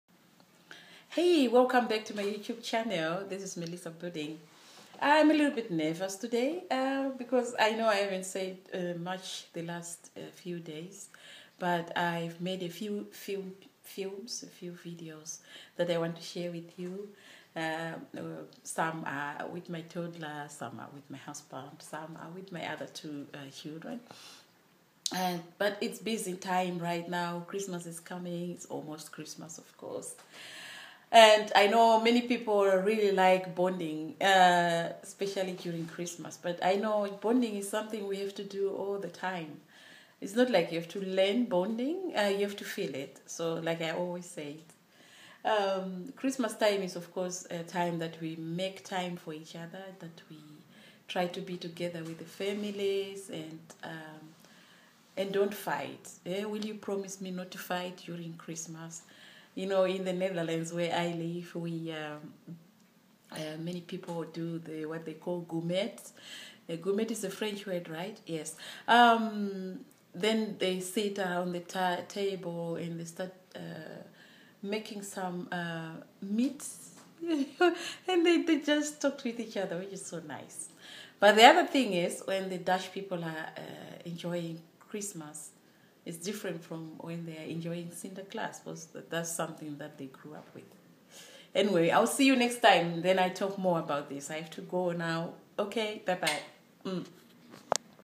Bonding becomes more important during Christmas?
The Dutch people really like it when you tell them in advance that you ware visiting, or joining diner at Christmas. They like preparing themselves. Mostly they are busy such that they wont see each other very often per year. Seeing each other less may affect the bonding in a negative way. Thats why being together during holidays has become important.